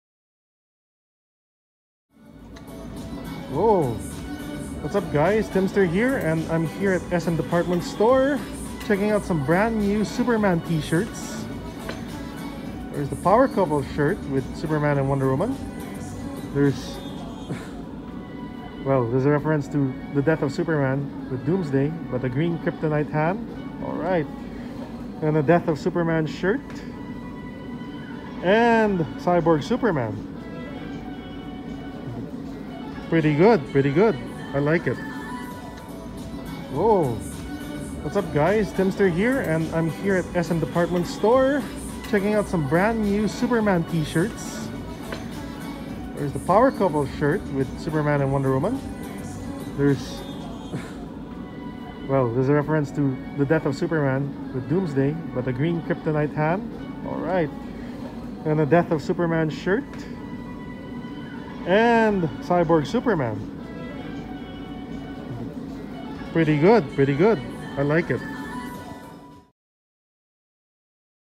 New Superman shirts at The SM Store! Death of Superman, Doomsday, Cyborg Superman, and more!
It’s the second month of 2021 and the Lunar Year celebration is close at hand, and what a perfect time to spot some brand new Superman shirts at the SM Department Store! Some classic designs and modern ones take center stage - particularly “Superman #75” or “The Death of Superman” issue, Superman and Wonder Woman, A Doomsday fist shirt holding Superman’s cape and Batman’s cowl, and finally Cyborg Superman! Definitely had to get some of these and it’s a must for any DC of fan of the Man of Steel! If you live in the Philippines, check your nearest SM Store now for these!